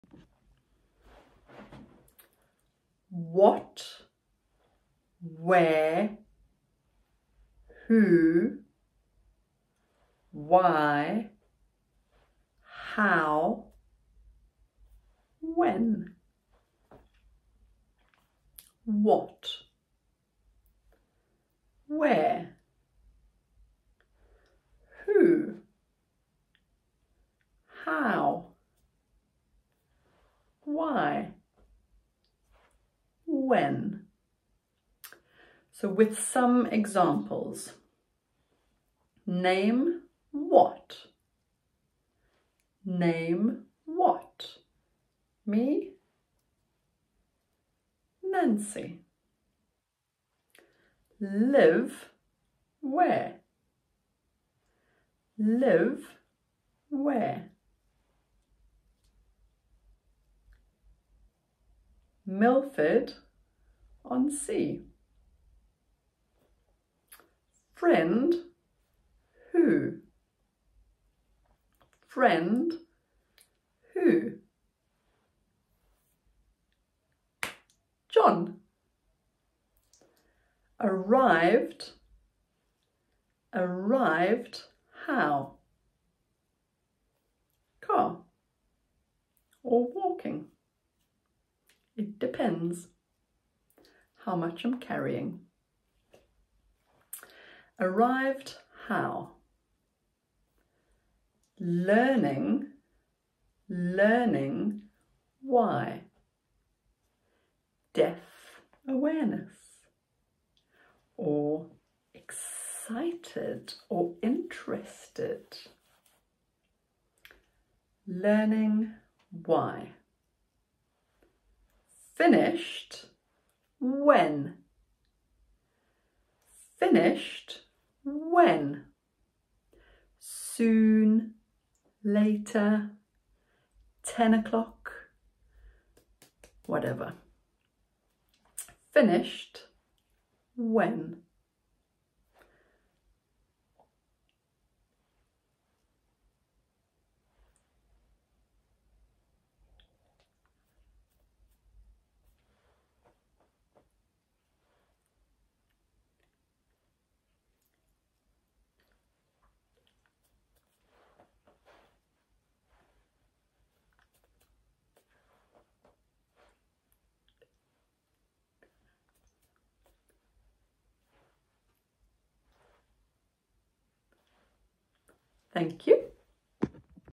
BSL what, where, when, why, how, who… - FeelHear Deaf Awareness and introduction to BSL
For my community - Milford on Sea, Hampshire